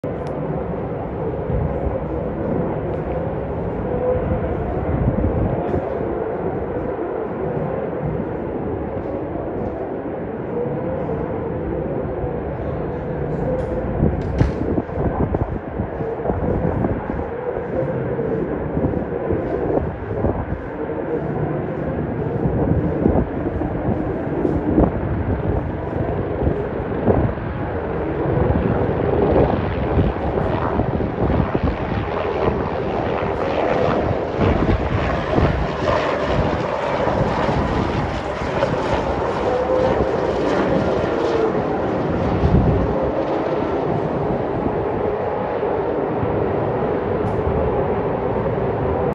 Helicopter flying close to skyscrapers on the Gold Coast
Helicopter flying between high rises above the Boostmobile Gold Coast 500